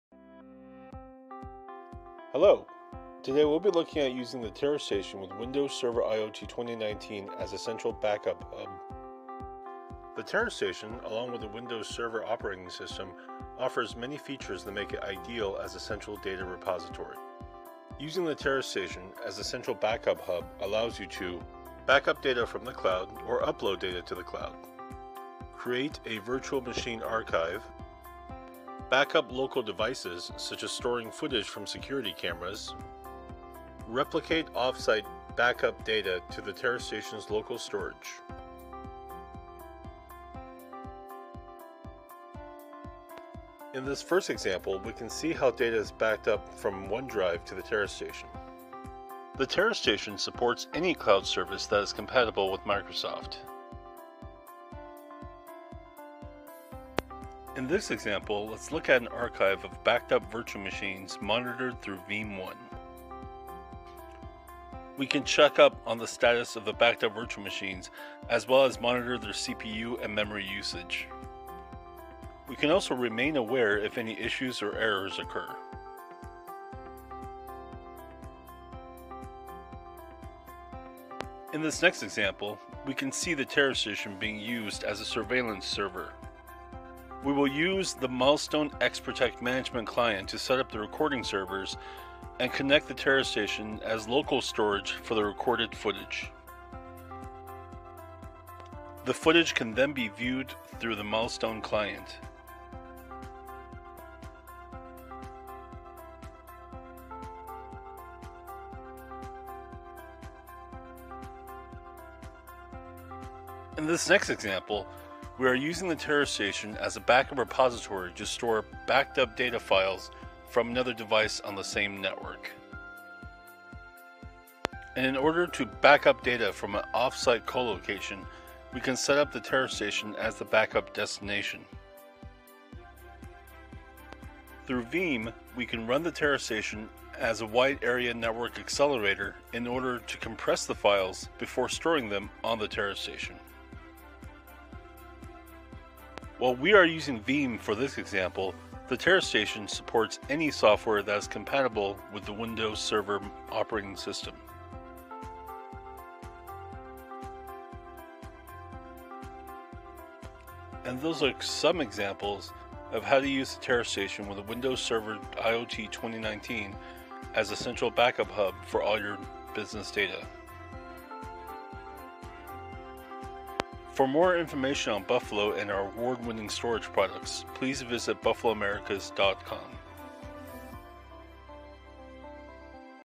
Using the Buffalo TeraStation NAS with Windows Server IoT as a Central Backup Hub
The security risks with cloud storage are well known, and managing a growing number of remote devices using cloud storage and backup alone can be dangerous and not to mention unwieldy.

Having a physical network attached storage device to serve as a central backup hub for all your cloud data can help you streamline remote work while enabling essential data security protection.